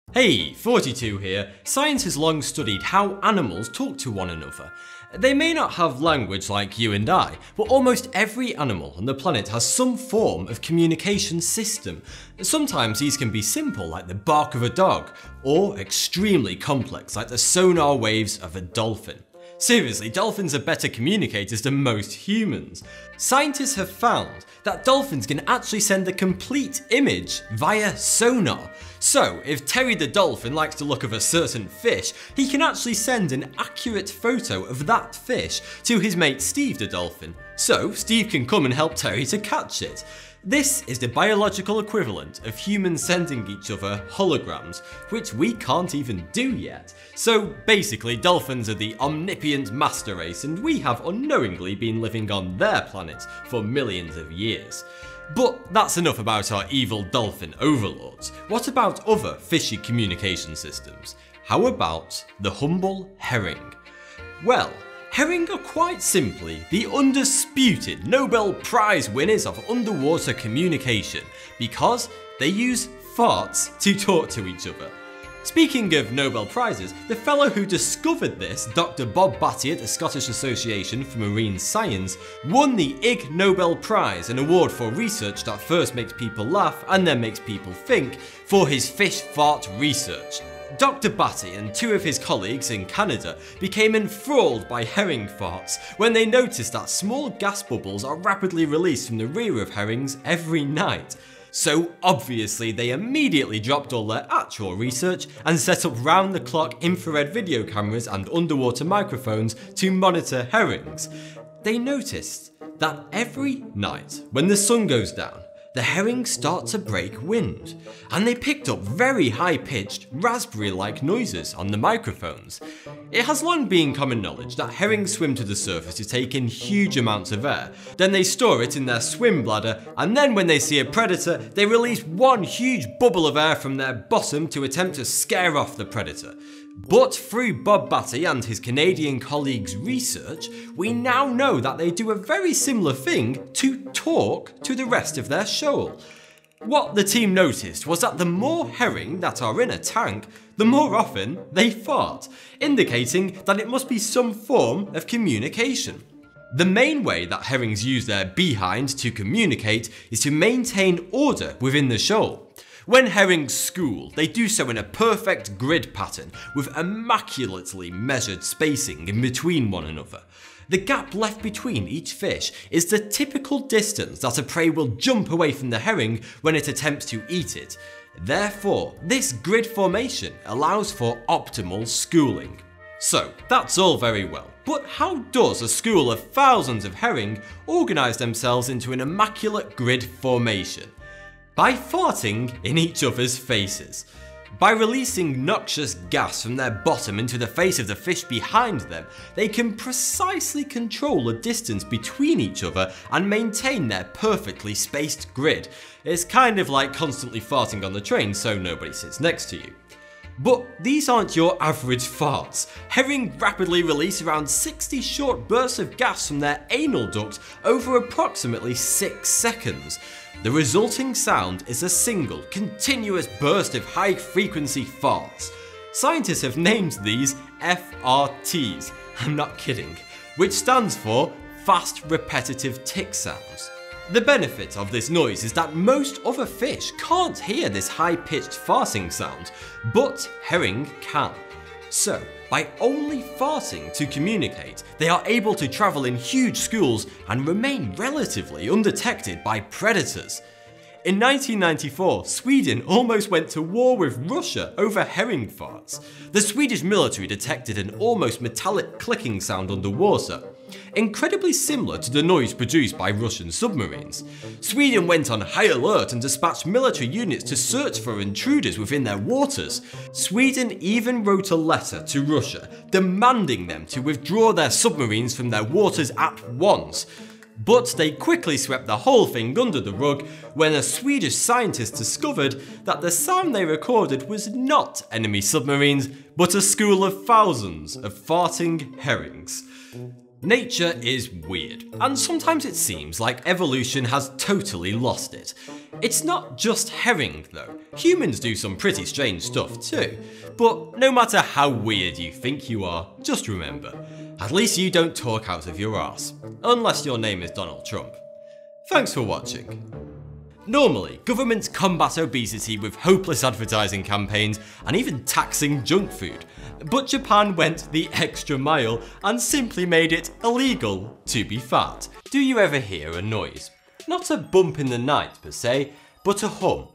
Herring Use FARTS to Talk to Each Other
With Special Thanks To:
Misha A-Wilson, Katrina Brogan, Michelle & Aaron Finn, Jeff Lee, Kent Zacherl, Steve Bradshaw, Lisa Pimlett, Matthew Russell, Saverius